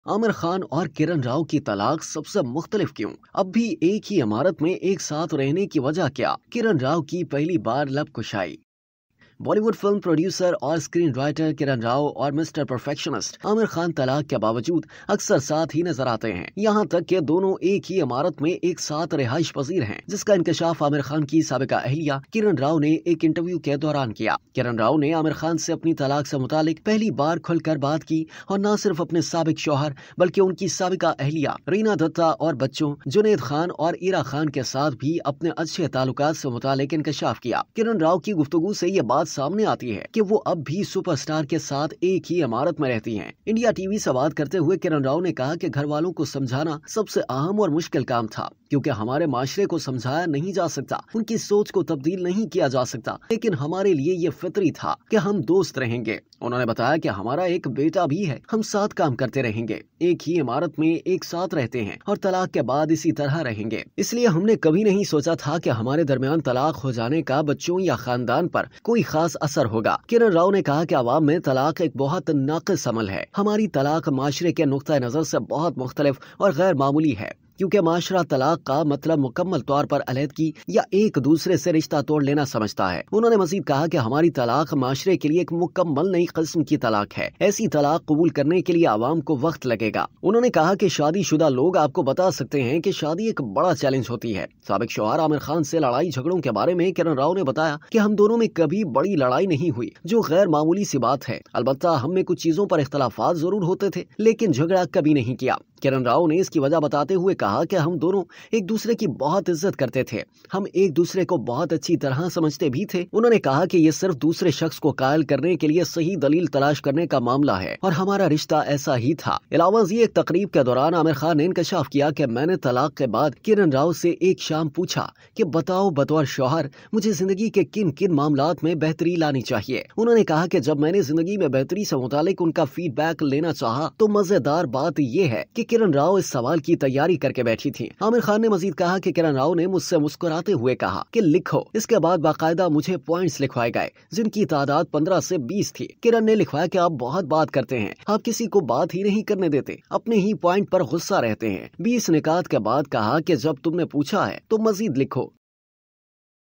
عامر خان اور کرن راؤ کی طلاق سب سے مختلف کیوں؟ | Aamir Khan | Kiran Rao | Bollywood
Bollywood film producer and screenwriter Kiran Rao and Mr. Perfectionist Aamir Khan are often seen together despite their divorce.